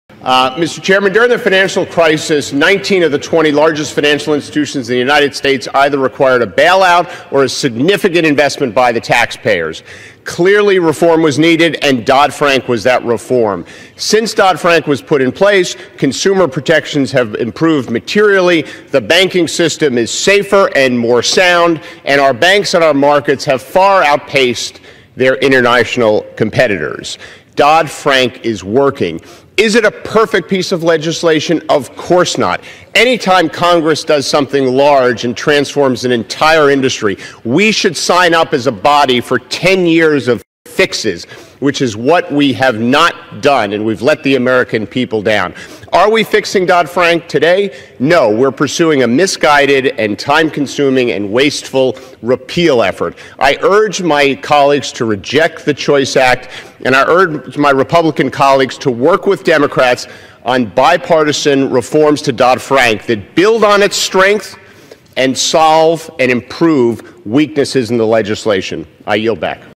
Rep. John Delaney Speaks Out Against Repealing Dodd-Frank
On June 8, 2017 the House of Representatives considered legislation to repeal and weaken significant provisions of the Dodd-Frank investor protections and financial reforms that were implemented after the financial crisis. Congressman John K. Delaney (MD-6) voted against the bill.